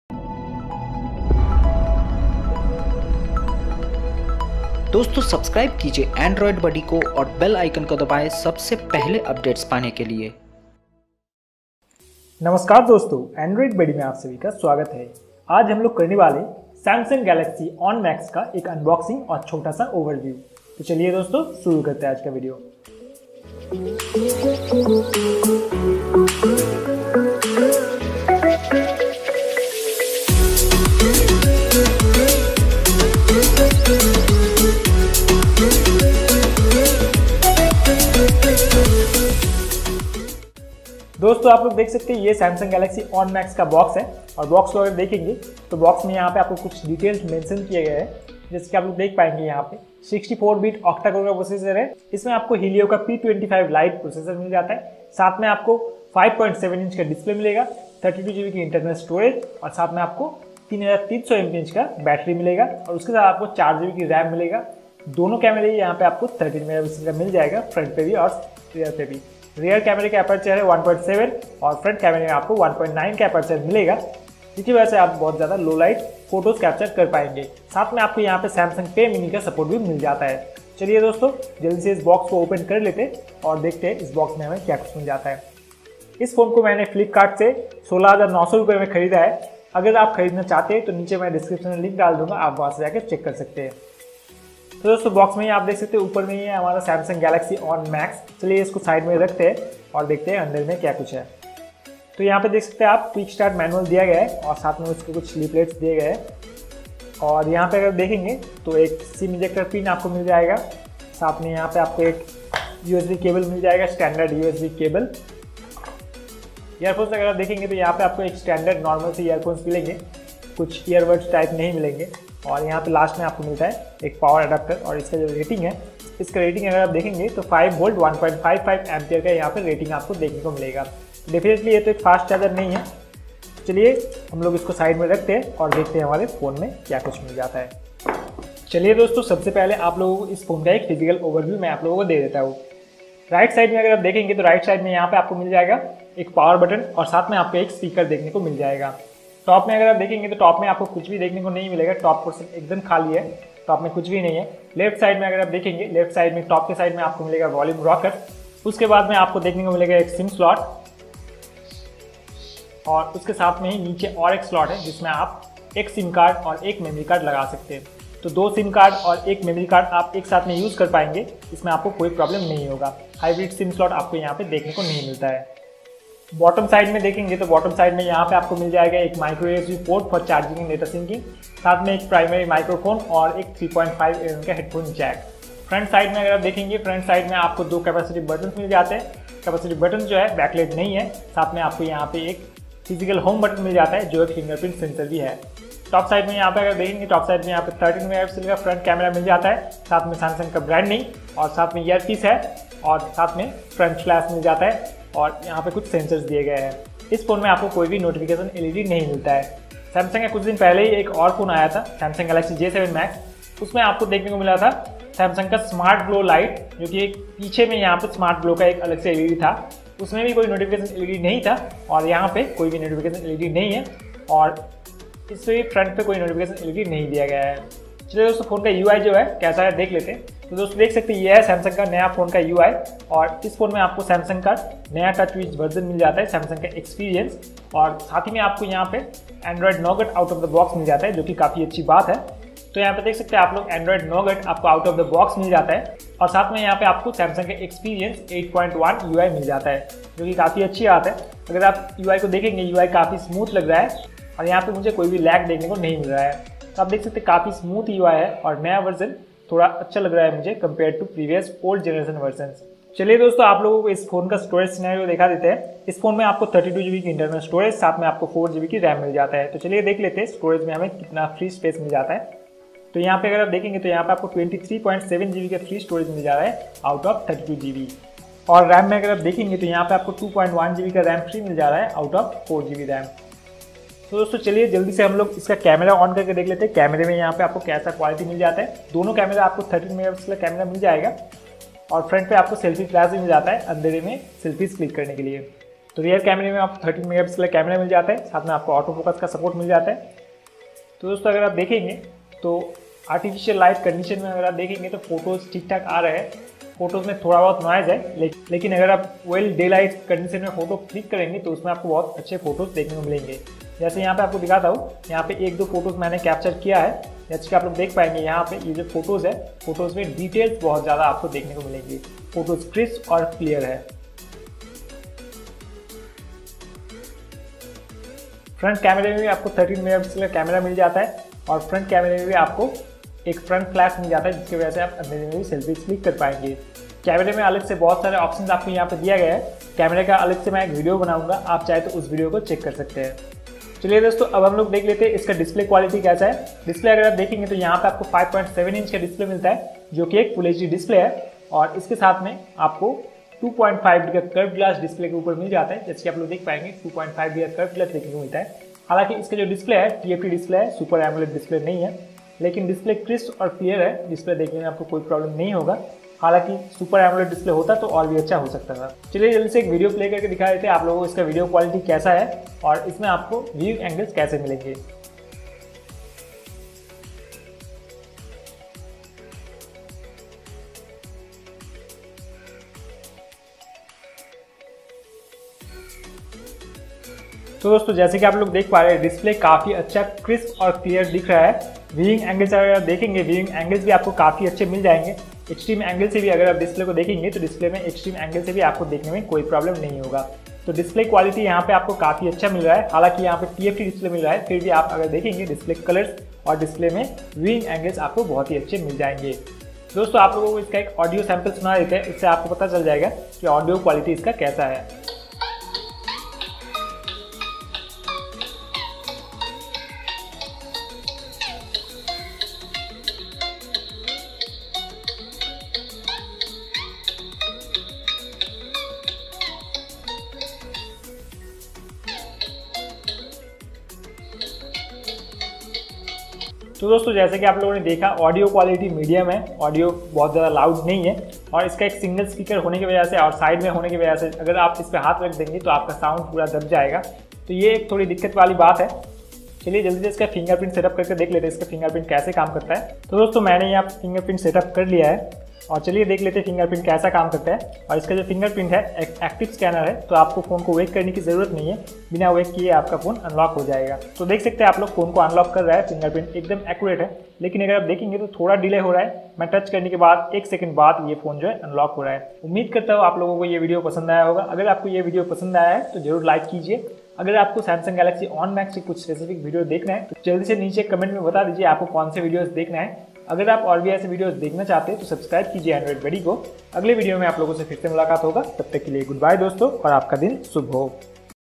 Samsung Galaxy On Max unboxing and overview in Hindi | Android Buddy
buy Samsung F23:

Friends here is the unboxing and overview of Samsung galaxy On max.Judwa bhai of J7 max

My Youtube gear